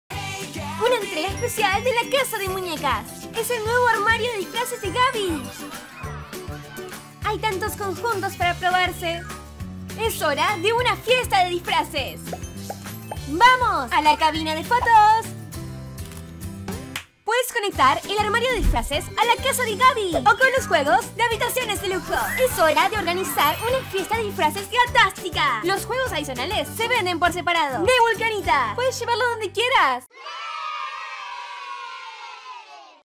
🐾 GABBY'S DOLLHOUSE - Nuevo Armario De Disfraces De Gabby 🙀
¡Miau miau! ¡Es una entrega de casa de muñecas! ¡El vestidor de Gabby! ¡Es hora de una fiesta de disfraces! Completa lindos looks para Gabby: ¡el atuendo de fiesta de baile, el atuendo de superhéroe y mucho más! Dale estilo a Gabby con accesorios espectaculares y divertite cambiandole el pelo. ¡Hacé que Gabby modele sus diferentes en la pasarela y que se admire en el espejo! Finalmente, dirígete a la cabina de fotos para tomarte algunas selfies. Cuando termines de jugar, simplemente cierra el armario y llévalo con vos donde quieras.

👉 ¡Conseguilo en tu juguetería más cercana!